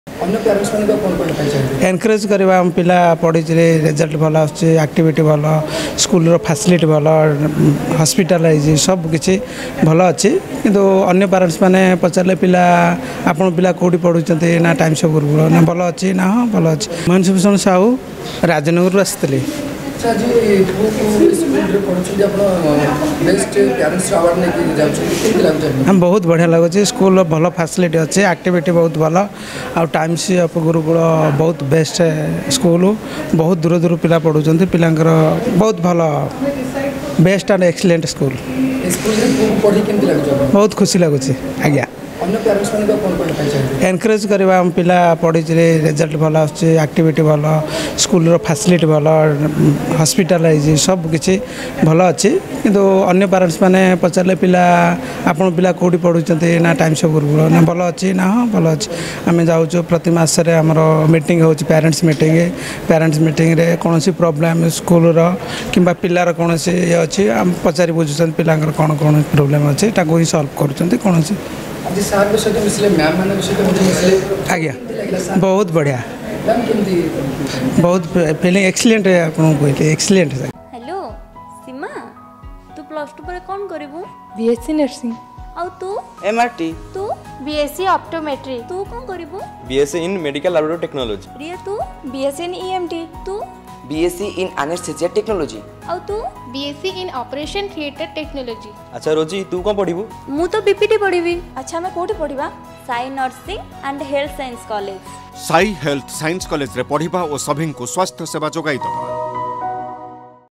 Tsg ଗୁରୁକୁଳ ସ୍କୁଲରେ ପଢ଼ିଲେ ଭଲ ରେଜଲ୍ଟ ଆସୁଛି | Tsg Gurukul School | Durabartta News
Tsg ଗୁରୁକୁଳ ସ୍କୁଲରେ ପଢ଼ିଲେ ଭଲ ରେଜଲ୍ଟ ଆସୁଛି ..